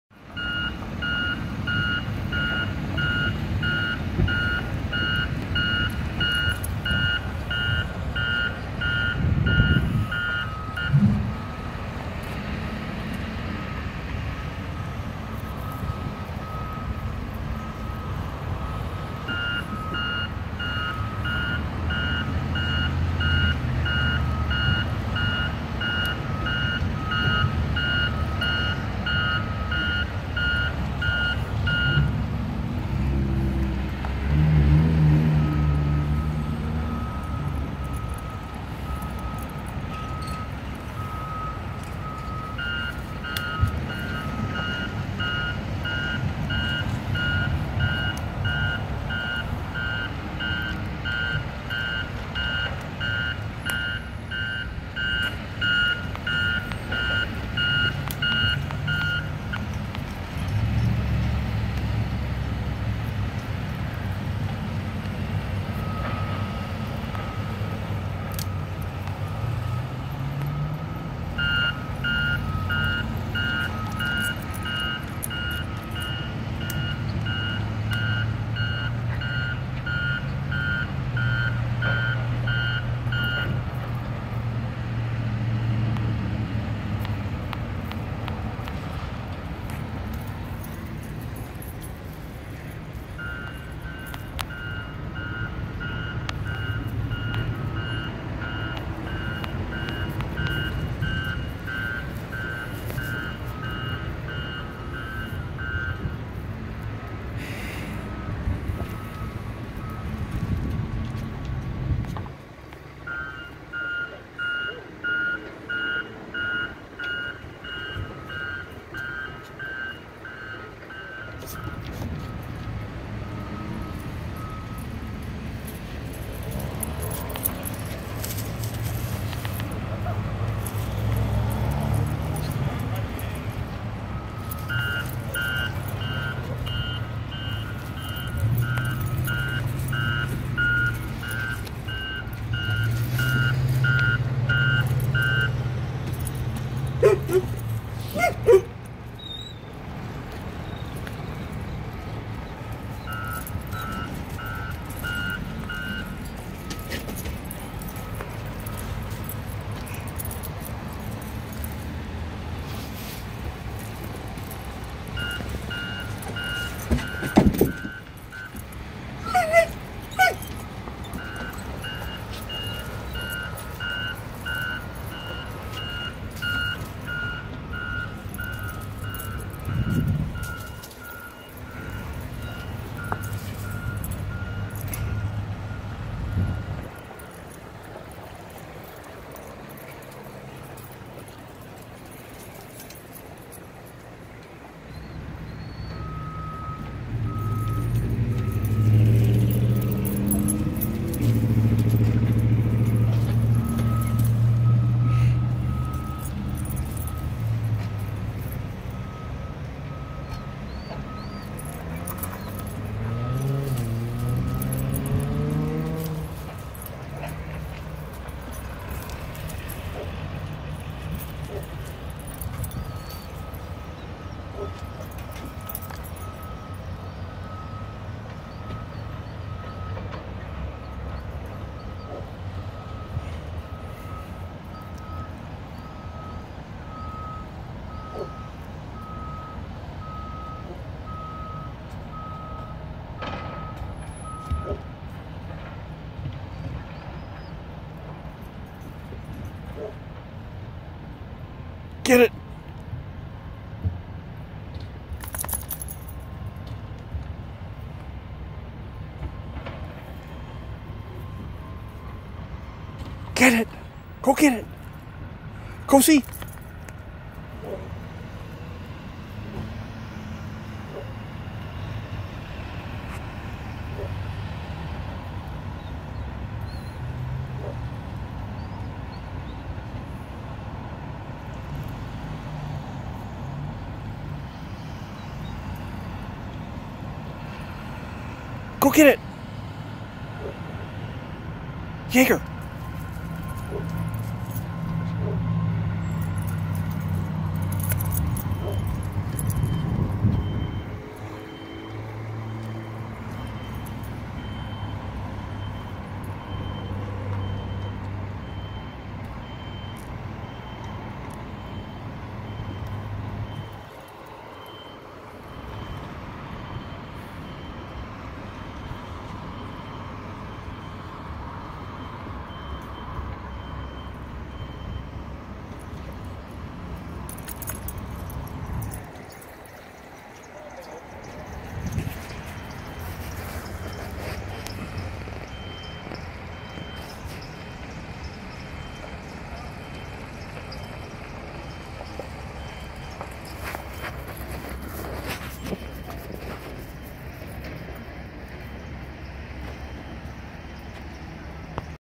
Jäger Entertains Himself
You should be able to tell there is construction in the area. That doesn't stop Jäger from playing in his yard.